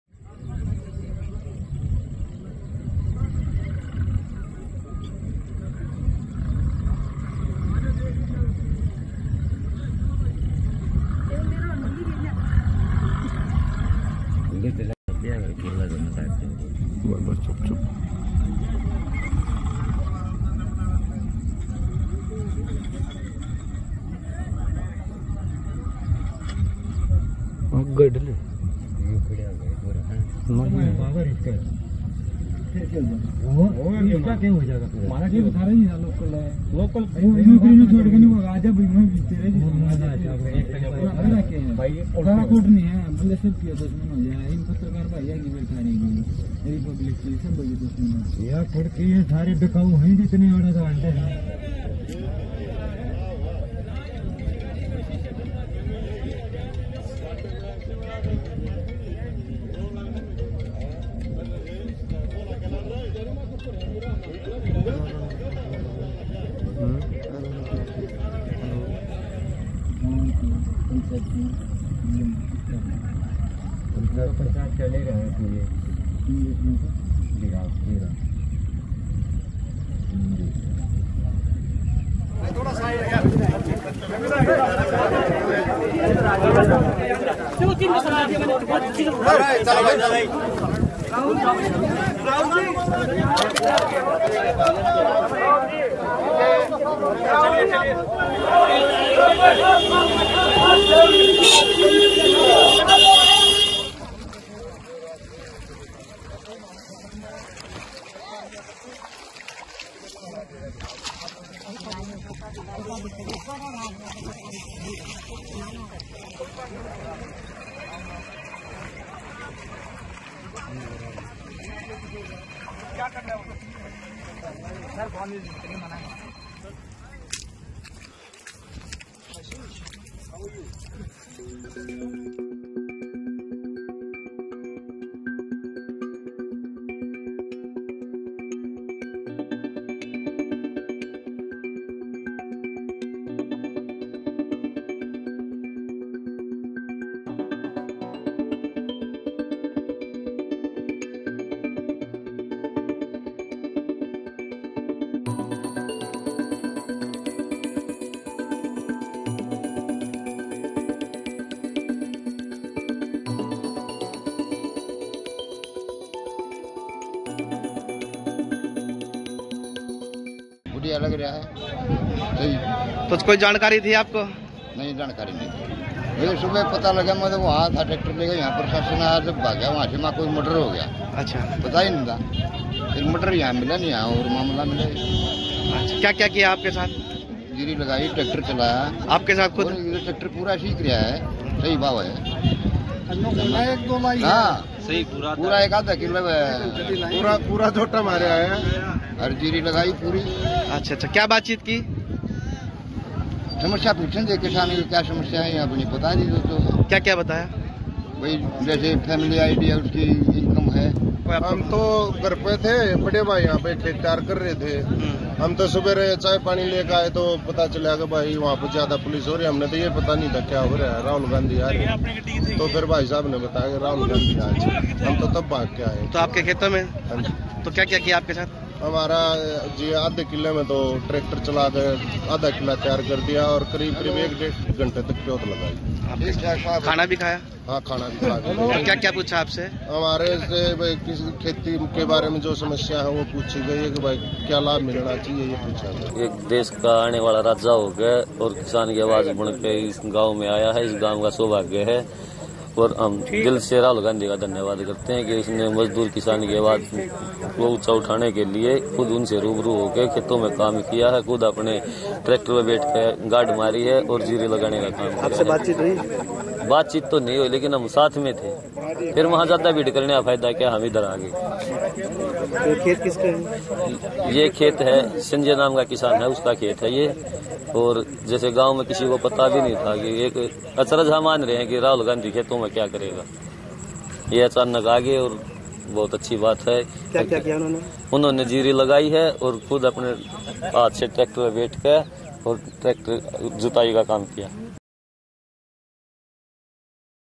Sonipat में अचानक खेतों में पहुंचे Rahul Gandhi, धान की रोपाई की, Rahul पर किसानों ने क्या कहा?
कांग्रेस(Congress) नेता राहुल गांधी(Rahul Gandhi) इन दिनों अलग-अलग अवतार में नजर आ रहे हैं. कुछ दिन पहले वह दिल्ली में एक बाइक मैकेनिक की दुकान में देखे गए थे और अब वह खेतों में नजर आए हैं. शनिवार सुबह- सुबह राहुल हरियाणा(Haryana) के सोनीपत (Sonipat)स्थित एक गांव में पहुंच गए और धान की रोपाई की. राहुल ने वहां ट्रैक्टर भी चलाया और किसानों से बात की.